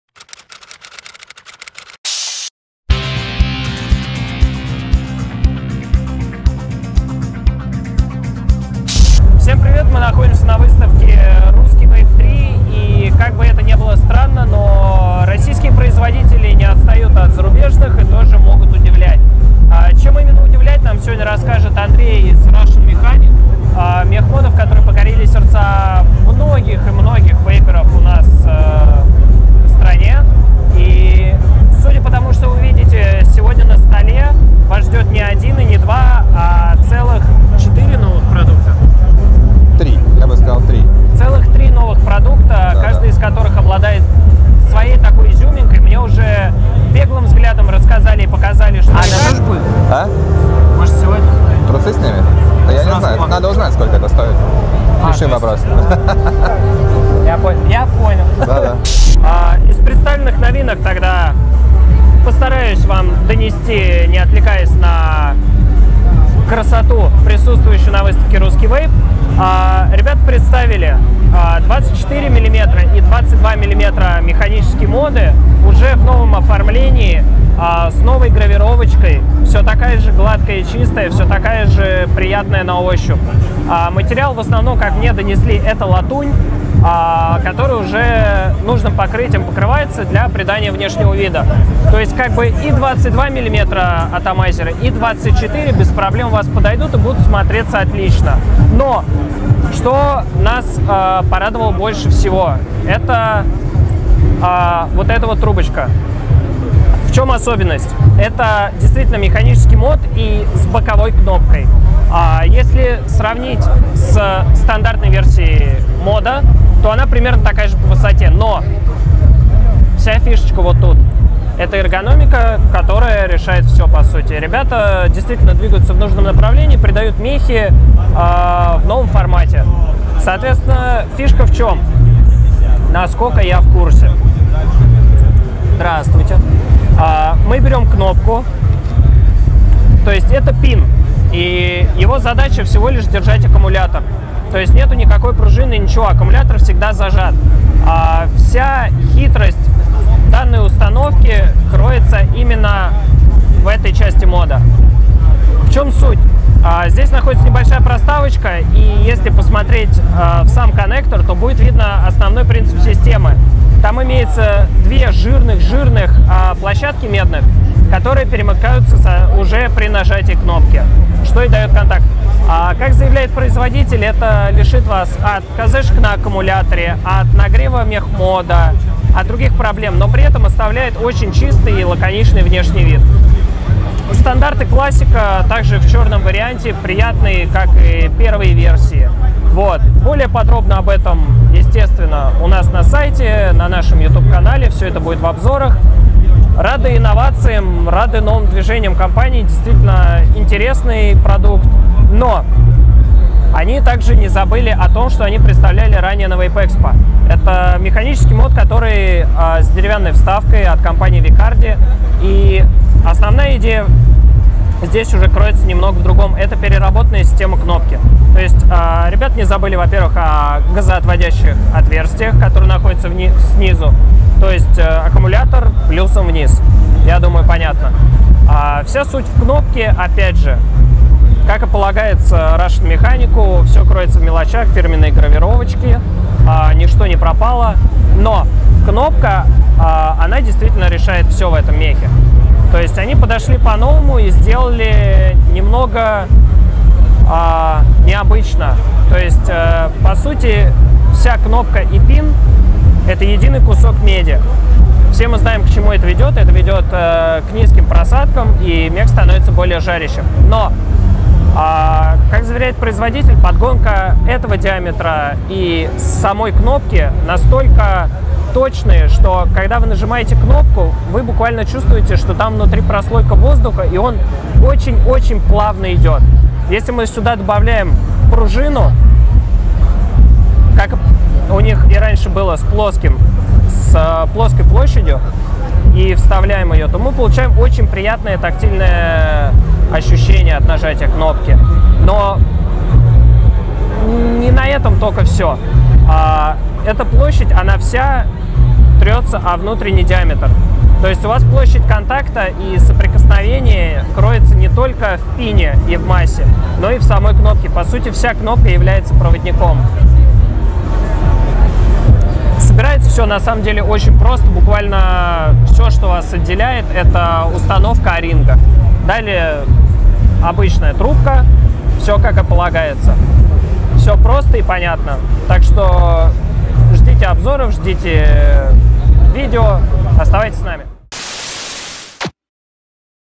Russian Mechanic @Russki Vape 3 | ViVA la Cloud
Поговорим о новинках от Vape Mechanic, которые представили обновленные девайсы на выставке RUSSKI VAPE III, прошедшей 23 и 24 сентября в Тесла 4000.